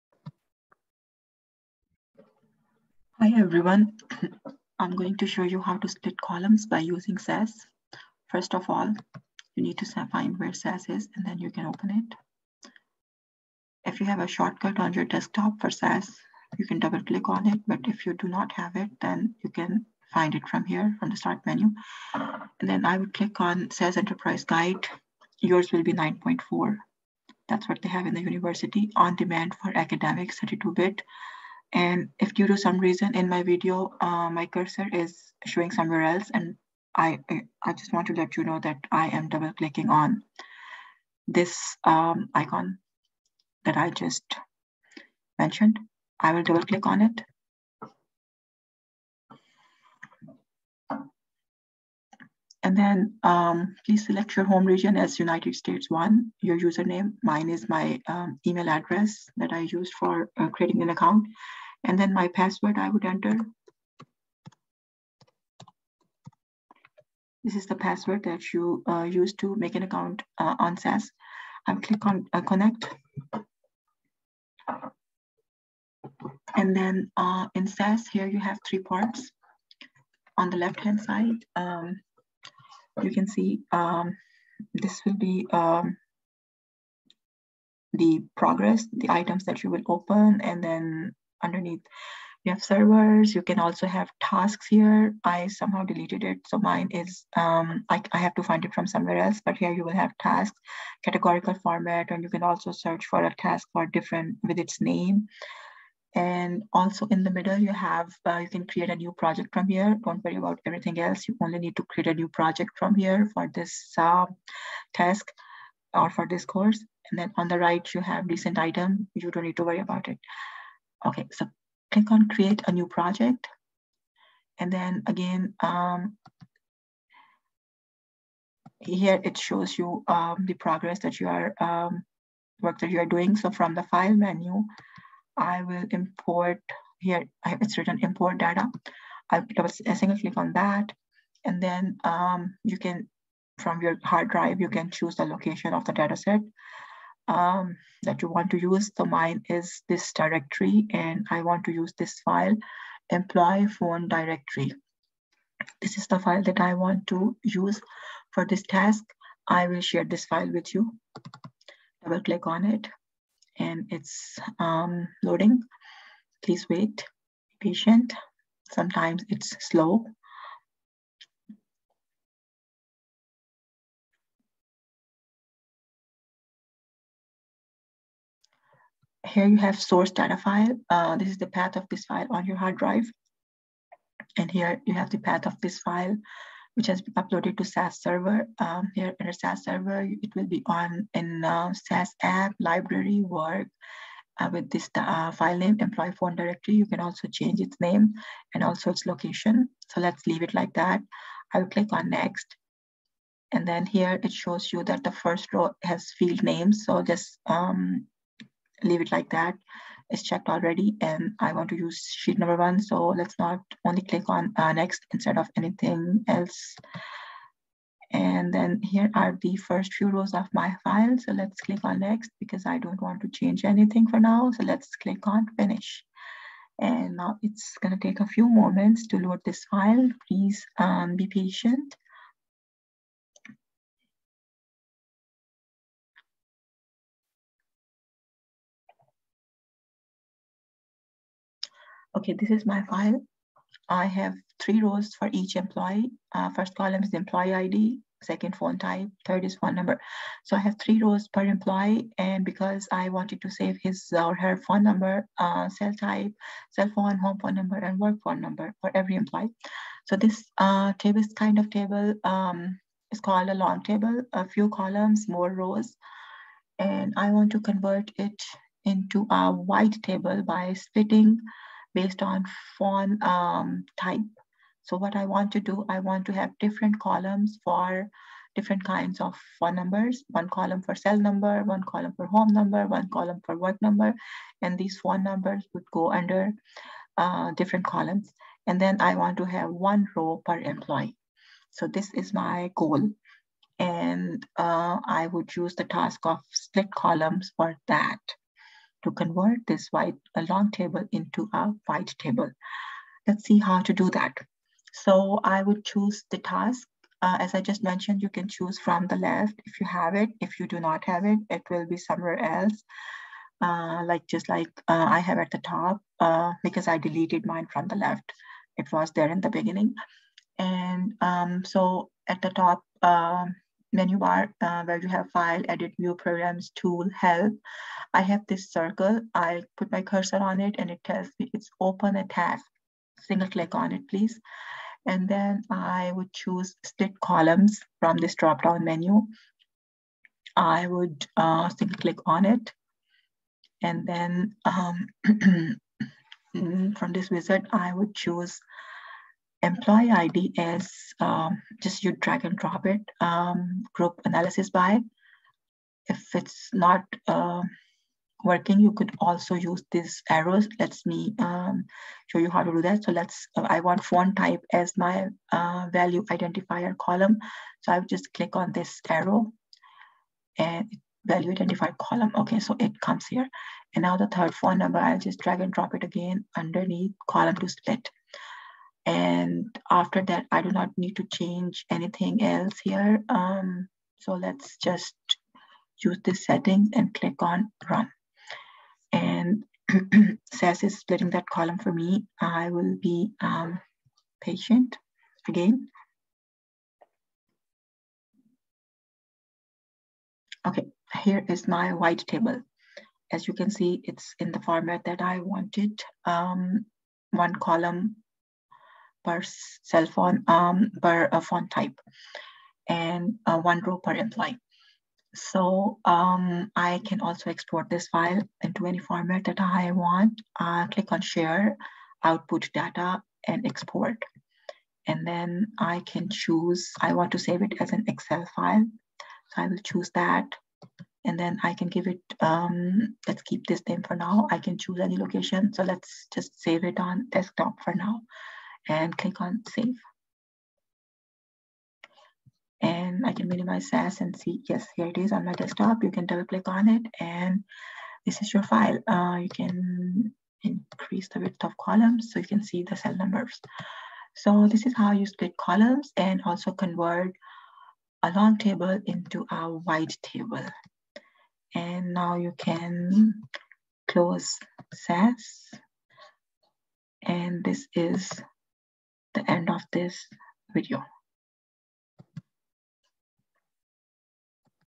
Split columns with SAS Enterprise Guide
Convert a long table into a wide table.